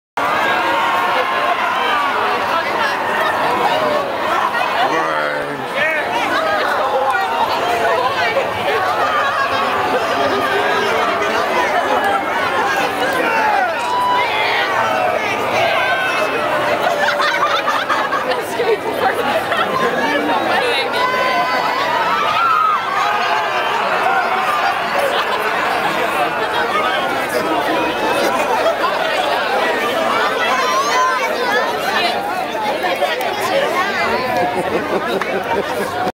Zombie Attack
the Nashville zombies are trying to eat the paprazzi!